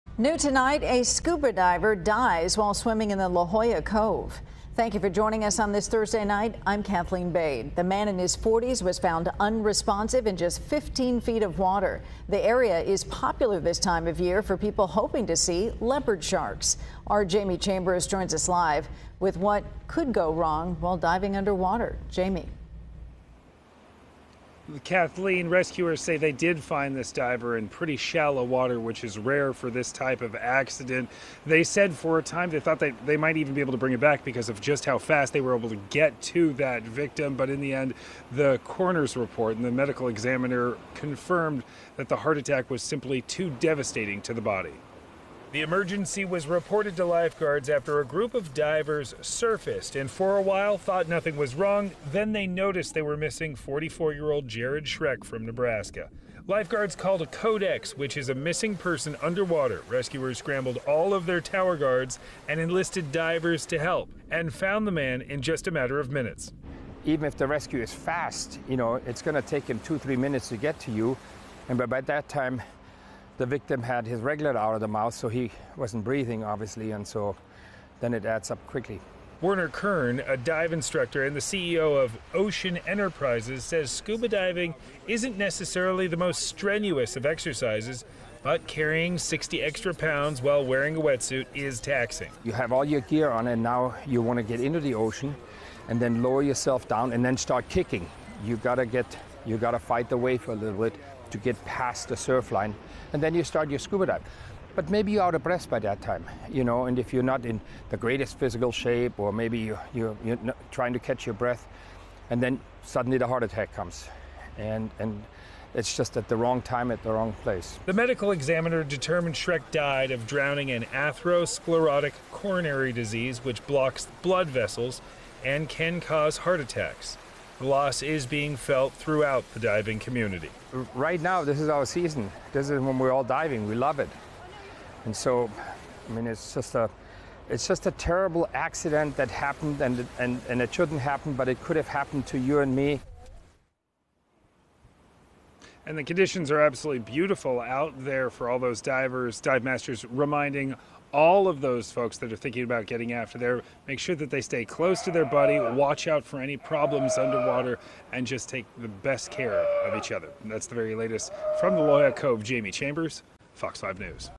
Scuba Diver Dies Off La Jolla Cove
A 44-year-old Nebraska man died while scuba diving off La Jolla Cove Sunday.